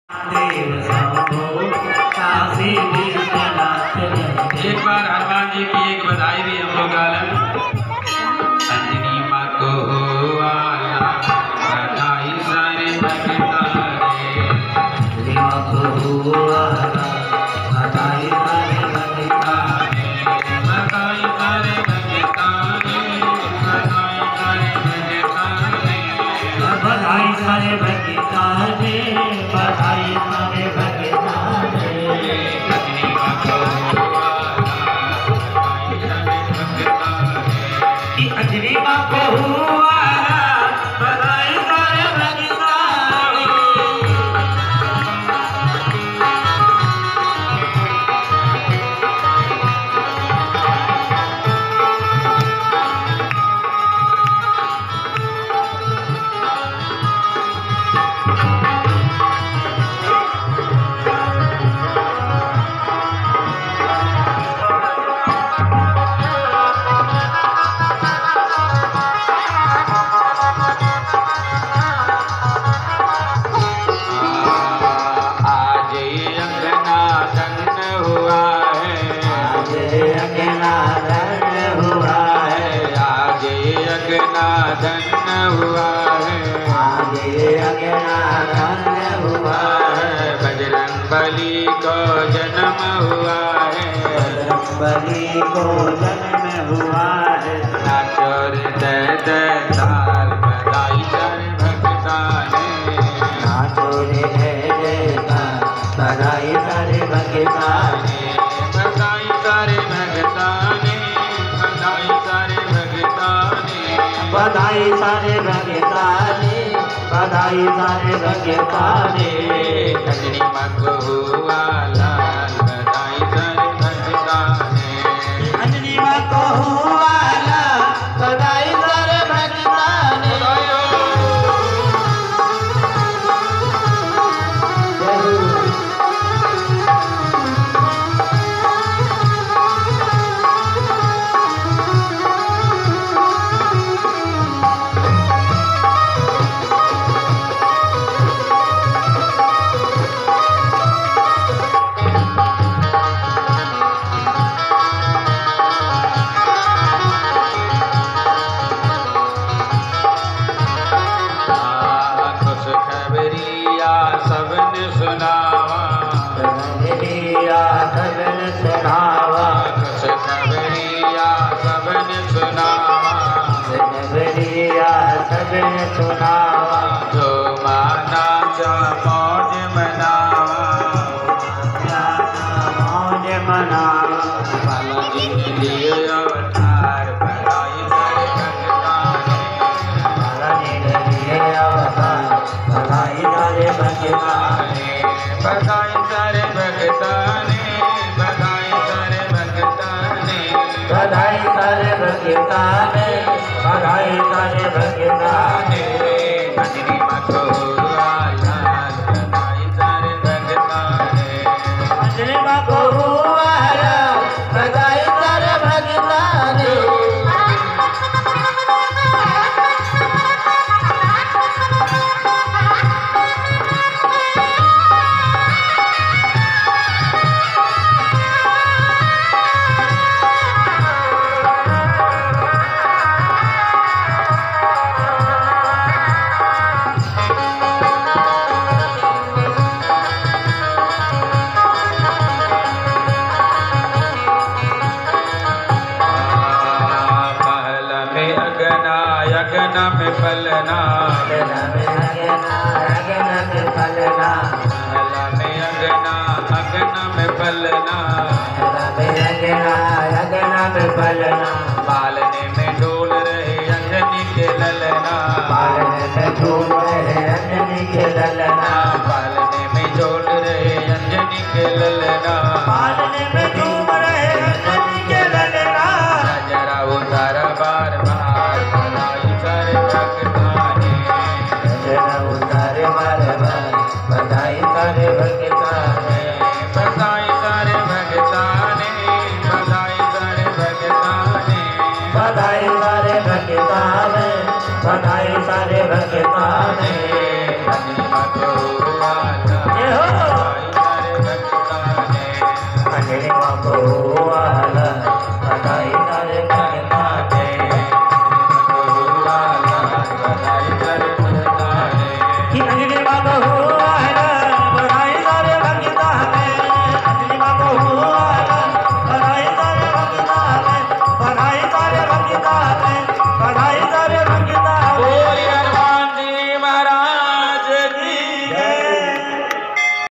श्री शिव महापुराण कथा में बधाई गाते हुए श्रद्धालु जन
@मानसरोवर गोरखपुर